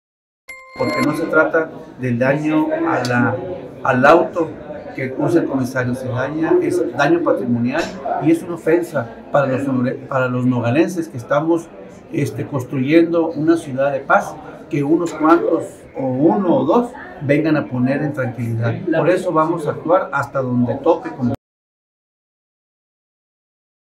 LE QUEMAN CARRO A COMISARIO
Las primeras horas del pasado 1 de abril, fue quemado de manera intencional, el vehículo que conduce el Comisario General de Seguridad Publica de Nogales, ante estos hechos la fiscalía realiza las indagaciones correspondientes, y la mesa de seguridad Sesiono de manera emergente este Domingo. El presidente municipal Juan Francisco Gim Nogales dio  a conocer que se tiene una investigación con un avance del 90 por ciento y envió a la comunidad el mensaje de que no se tolerara ningún hecho que atente contra el municipio, y mucho menos la ciudadanía. Juan Francisco Gim Nogales dio a conocer que en el transcurso del día se darían a conocer los avances respecto a la investigación y enfatizo que no se tiene ninguna amenaza a la corporación incluso el Comisario desarrollo este lunes su trabajo de manera normal, el alcalde indico que toda la “fuerza del estado”, vela por la tranquilidad de la ciudadanía Nogalense.